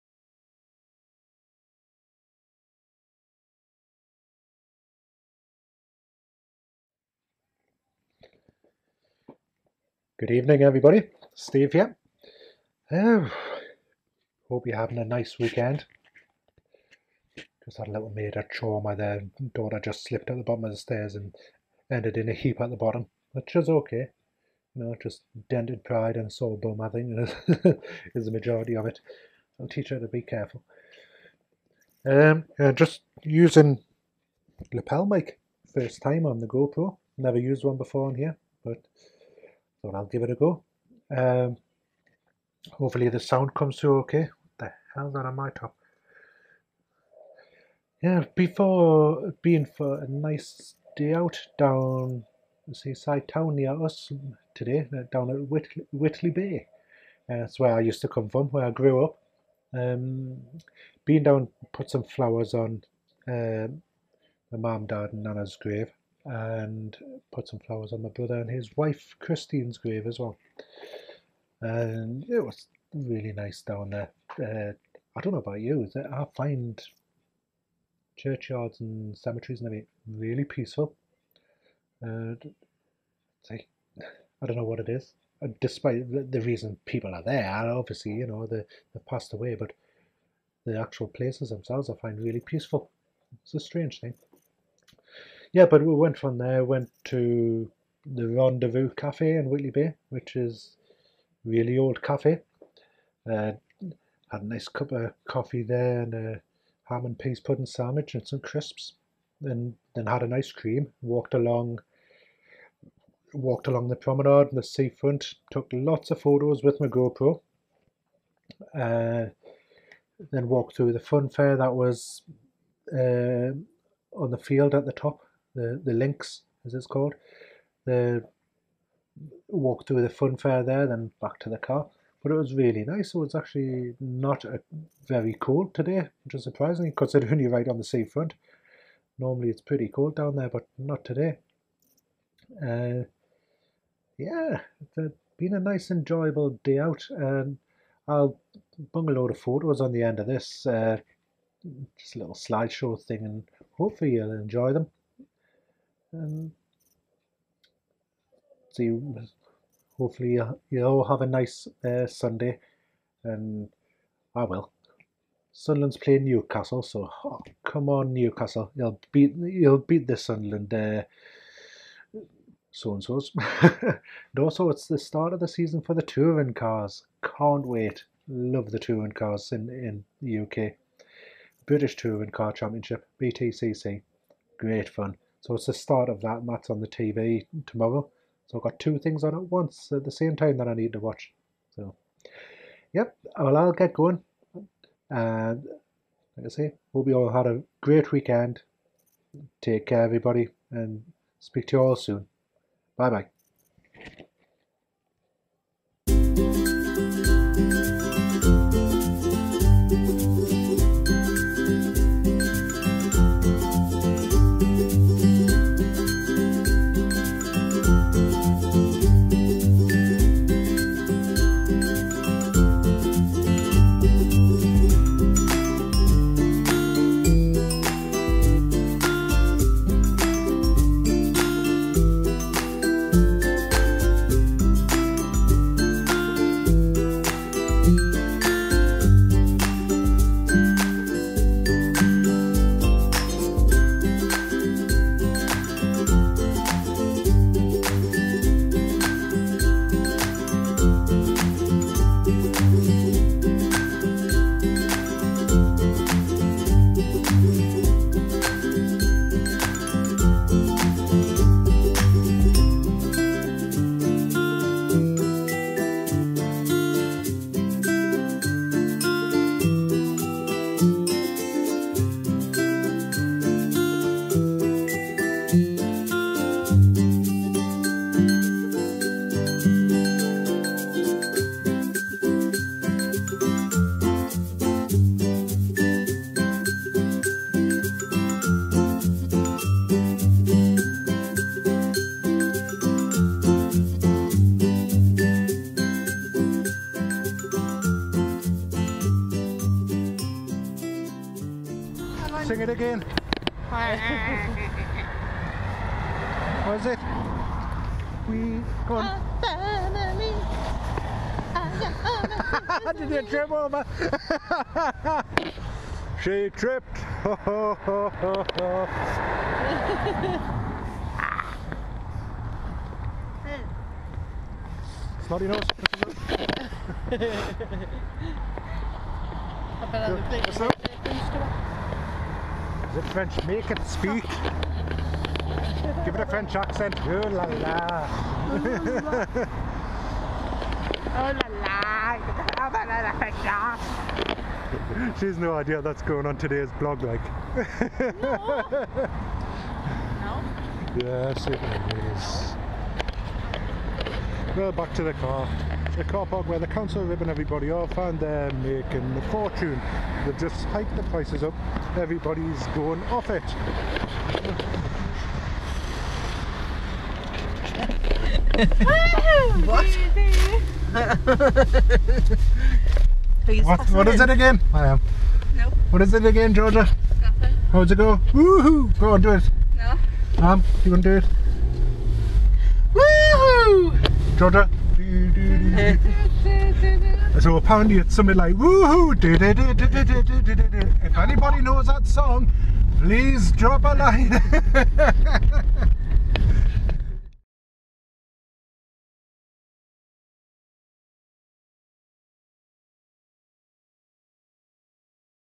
Trip to the coast ( Vlog #6)
A trip to the coast at Whitley Bay.